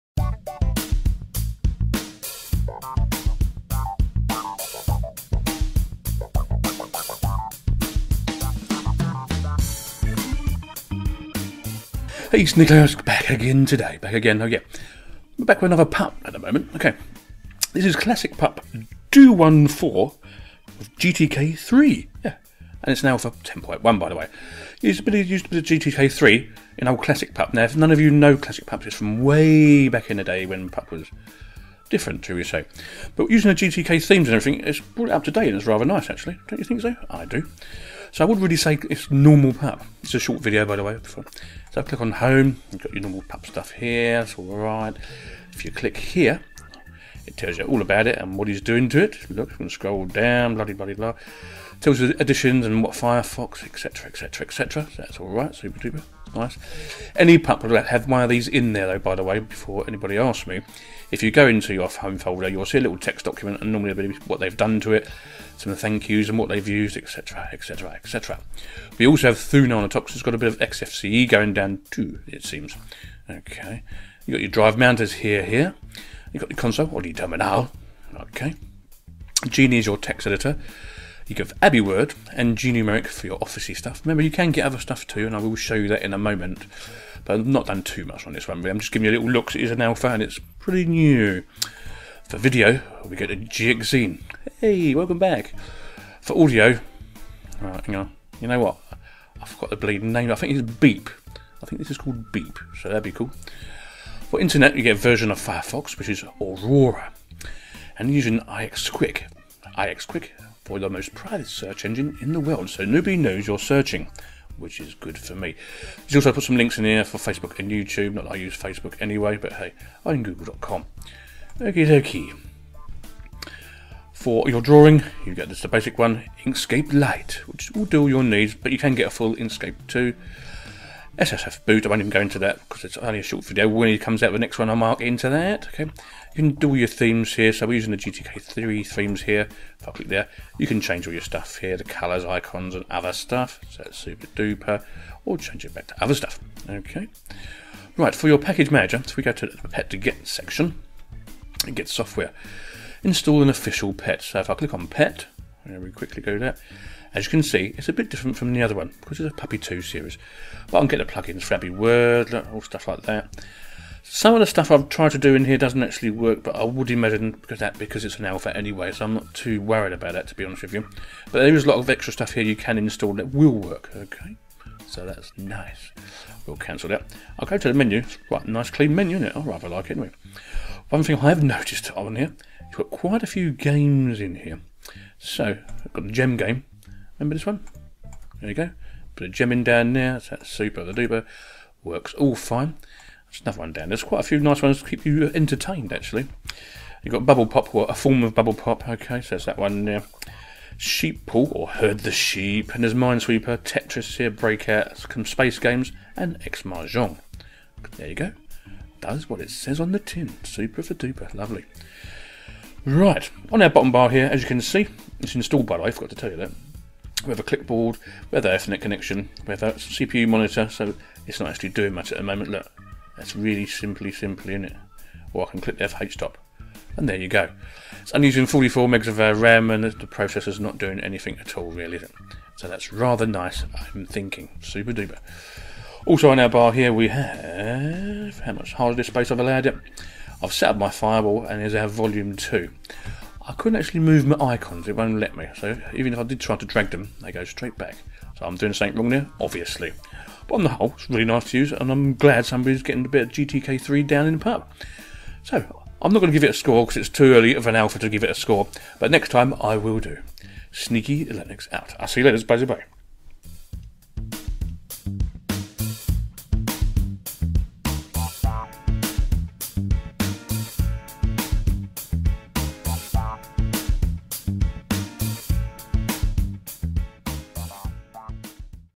Classic Pup 214 GTK3 Alpha 10.1...Long Title.!!
Here we have a little project from tuxxxx and it's classic pup 214 gtk 3, so he is having a play with gtk 3 to see how it goes and it does look quite good and for the size of the d/l it is worth you time to have a look at it...super